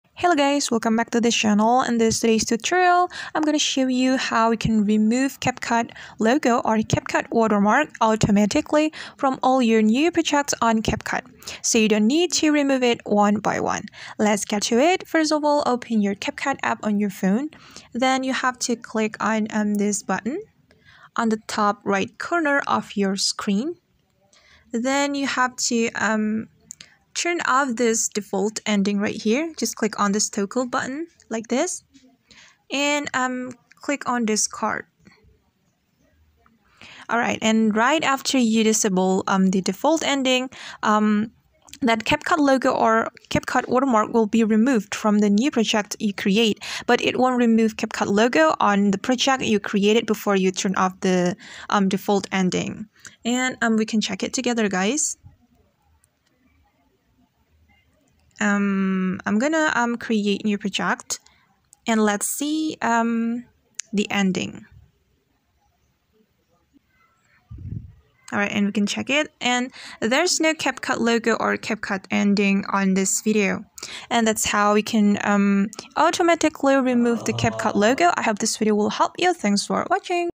No Need to Delete One by One! How to Permanently Remove CapCut Ending from All New Projects?
You may forget to delete CapCut ending which automatically added to our projects at some point. As a consequence, you have to re-export your projects. Don't worry guys, we've got you covered. In this tutorial, we're gonna learn how to disable CapCut logo automatically!